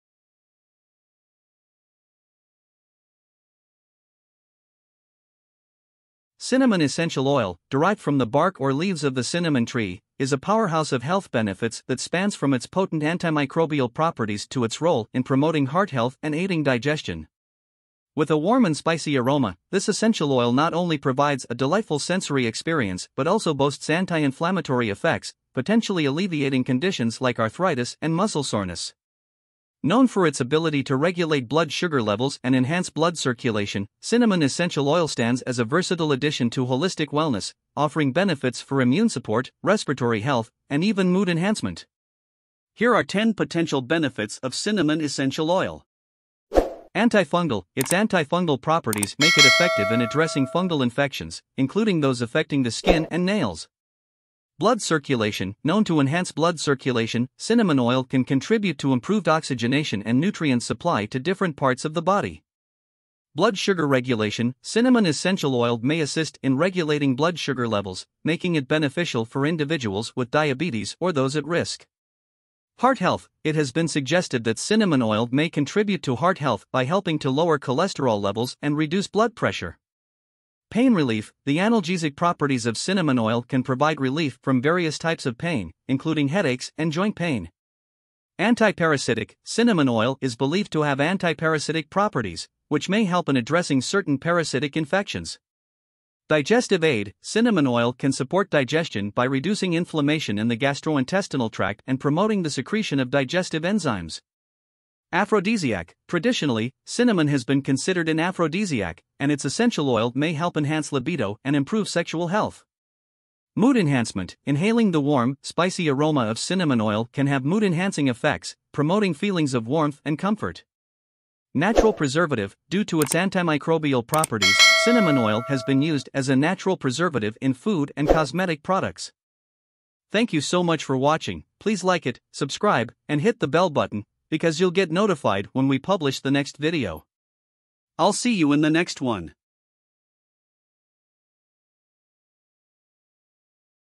10 BENEFITS OF CINNAMON ESSENTIAL OIL
Cinnamon essential oil is renowned for its numerous health benefits. This aromatic oil, extracted from cinnamon bark, possesses anti-inflammatory, antioxidant, and antimicrobial properties, making it a valuable ally in bolstering the immune system and combatting infections. Furthermore, it can aid in regulating blood sugar levels and improving circulation. Its warm and comforting aroma is also used in aromatherapy to alleviate stress and promote a sense of well-being. However, it is important to use cinnamon essential oil in moderation, as its potency can lead to skin irritation when applied directly and is not recommended during pregnancy.